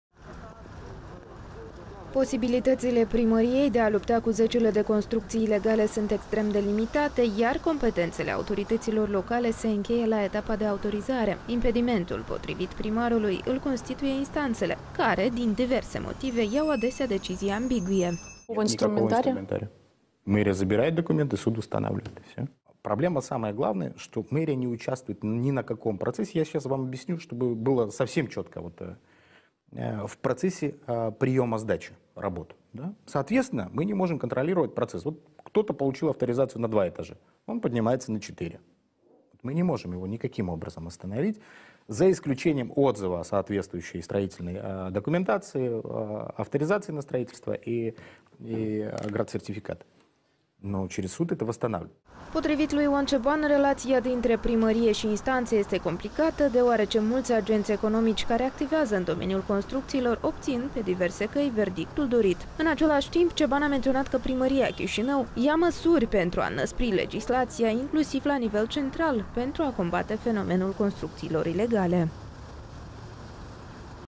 Război construcțiilor neautorizate
De când a preluat funcția de primar general al municipiului Chișinău, nu a fost emisă nicio autorizație pentru construcțiile ilegale. Asta declară chiar Ion Ceban, care a precizat că aceasta a fost și una dintre promisiunile sale din campania electorală, pe care le pune în practică. Ceban mai spune că aproximativ 83% dintre aceste autorizații au fost emise de către predecesorii săi, iar autoritățile locale simt și astăzi povara respectivelor fărădelegi.

Posibilitățile Primăriei de a lupta cu zecile de construcții ilegale sunt extrem de limitate, iar competențele autorităților locale se încheie la etapa de autorizare. Impedimentul, potrivit primarului, îl constituie instanțele care, din diverse motive, iau adesea decizii ambigue.

Ion Ceban, primar general al Chișinăului

”Primăria nu dispune de instrumente în acest sens. Primăria retrage actele, instanța le restabilește. Cea mai mare problemă este că Primăria nu este implicată în procesul de primire-predare, prin urmare, nu putem controla procesul. Cineva a primit autorizație pentru două  etaje, dar construiește o clădire cu patru nivele. Nu-l putem opri în niciun fel, cu excepția revocării autorizației de construcție și a certificatului de urbanism. Dar prin instanță, aceste acte sunt restaurate.”

Potrivit lui Ion Ceban, relația dintre Primărie și instanțe este complicată, deoarece mulți agenți economici care activează în doemniul construcțiilor obțin,  pe diverse căi, verdictul dorit. În același timp, Ceban a menționat că Primăria Chișinău ia măsuri pentru a înăspri legislația, inclusiv la nivel central, pentru a combate fenomenul construcțiilor ilegale.